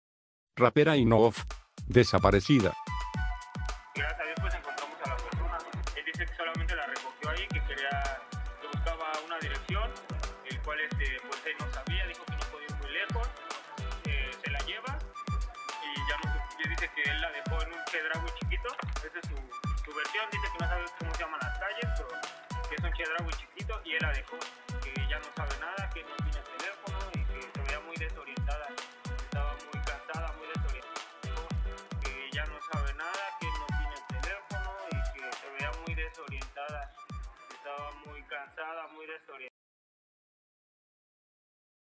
Rapera Inof habla su hermano del mototaxi en Ecatepec
Mototaxista dice que dejó a la rapera Inof en Chedraui de Ecatepec. POR FAVOR, AYÚDENOS Q ENCONTRAR A INOF.  Se encuentra perdida  rapera Inof en Ecatepec tras subirse a un mototaxi. Su hermano habla.